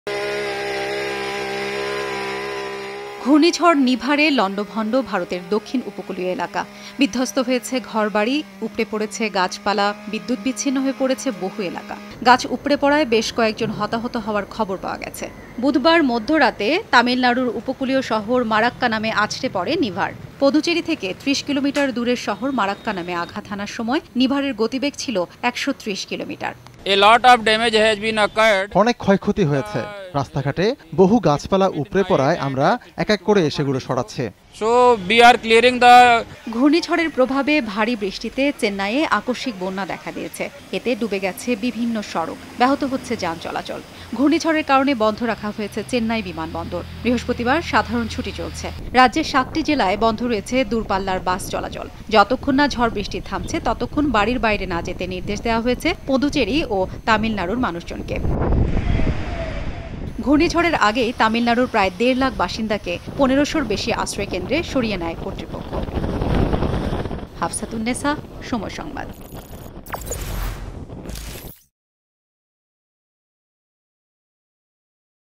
শক্তি হারাচ্ছে নিভার, চেন্নাই ও পুদুচেরিতে বৃষ্টি | Cyclone Nivar | Tamil Nadu Chennai | Puducherry
ঘূর্ণিঝড় নিভারে লণ্ডভণ্ড ভারতের দক্ষিণ উপকূলীয় এলাকা। বিধ্বস্ত হয়েছে ঘর-বাড়ি, উপড়ে পড়েছে গাছপালা, বিদ্যুৎবিচ্ছিন্ন হয়ে পড়েছে বহু এলাকা। গাছ উপড়ে পড়ায় বেশ কয়েকজন হতাহত হওয়ার খবর পাওয়া গেছে। বুধবার রাত আড়াইটার দিকে তামিলনাড়ুর উপকুলীয় শহর মারাক্কানামে আছড়ে পড়ে নিভার। পদুঁচেরি থেকে ৩০ কিলোমিটার দূরের শহর মারাক্কানামে আঘাত হানার সময় নিভারের গতিবেগ ছিলো ১শ ৩০ কিলোমিটার। ঘূর্ণিঝড়ের প্রভাবে ভারী বৃষ্টিতে চেন্নাইয়ে আকস্মিক বন্যা দেখা দিয়েছে, এতে ডুবে গেছে বিভিন্ন সড়ক, ব্যাহত হচ্ছে যান চলাচল।
Landabhand is a southern coastal area of ​​India in cyclone shelter. Houses have been destroyed, trees have been uprooted, many areas have been cut off from electricity. Several people were reported killed when the tree fell. Nivar crashed in the coastal town of Marakkanam in Tamil Nadu around 2.30 pm on Wednesday. Nivar was traveling at a speed of 130 kmph when it struck Marakkanam, a town 30 km from Padunchery. The cyclone has caused flash floods in Chennai due to heavy rains, inundating various roads and disrupting traffic.